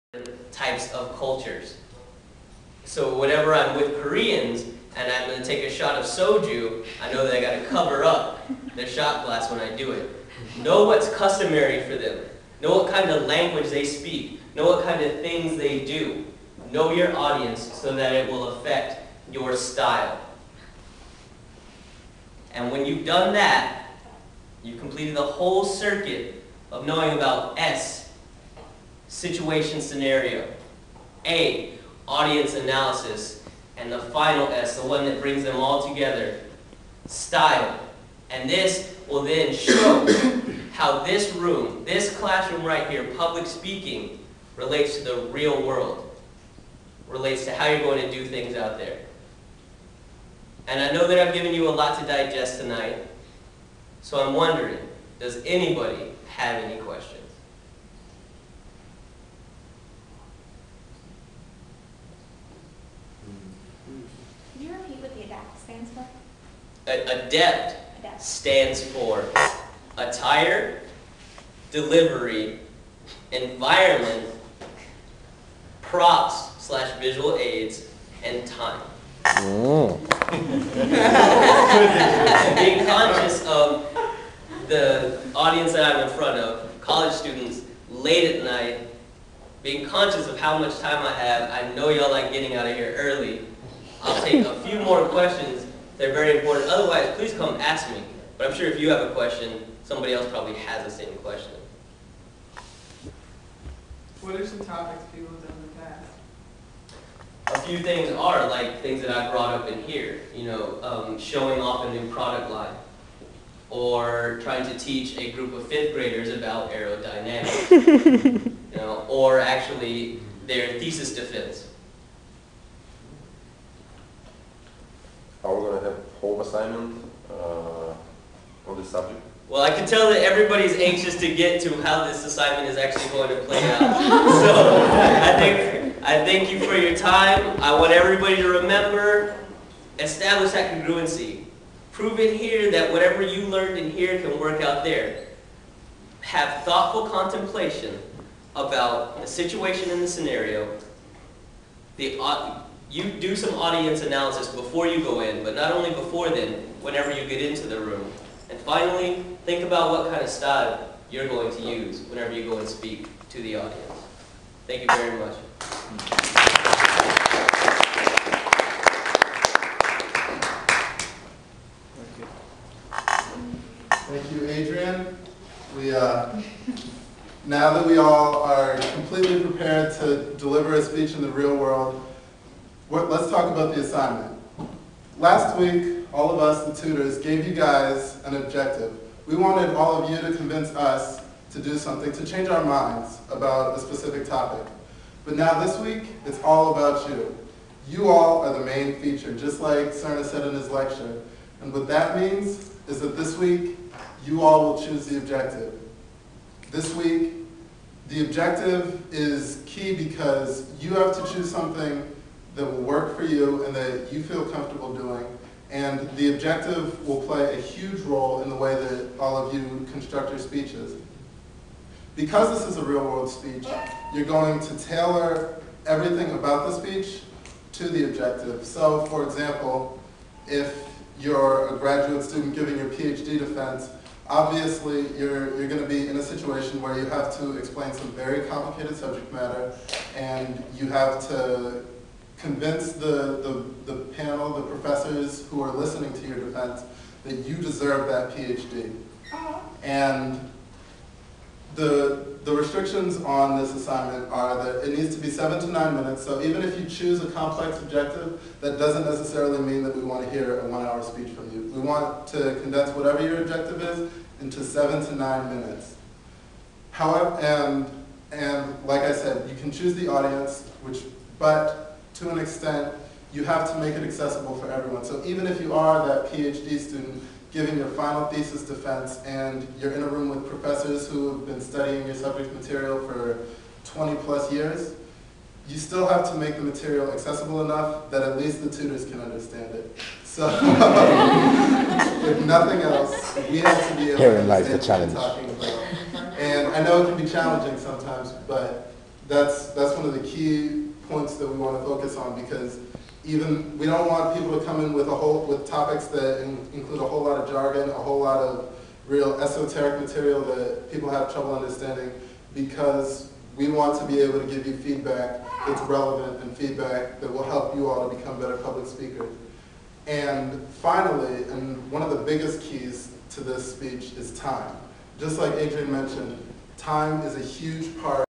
Serna - Professional Speech #2
This is a lecture from the Stanford course Engineering 103 (E103). It's about how to give a professional/real world speech. It is given by Josue Adrian Serna, one of the Course Captains. John Dryden comes in at the end to handle the instructions for the next assignment. The big hand is courtesy of the other Course Captain Kent Casimir